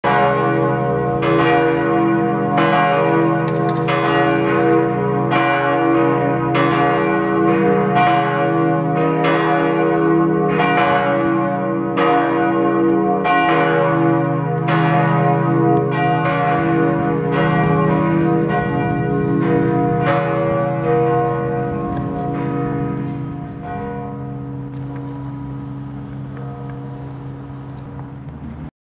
Karlstads domkyrka, slutet av helgmålsringning
Slutet av helgmålsringning i Karlstads domkyrka, på sensommaren 2015. Här på klippet ringer domkyrkans alla tre klockor och jag zoomar in från nordost och Mellanklockan syns röra sig. Jag skapade det här videoklippet med YouTubes videoredigerare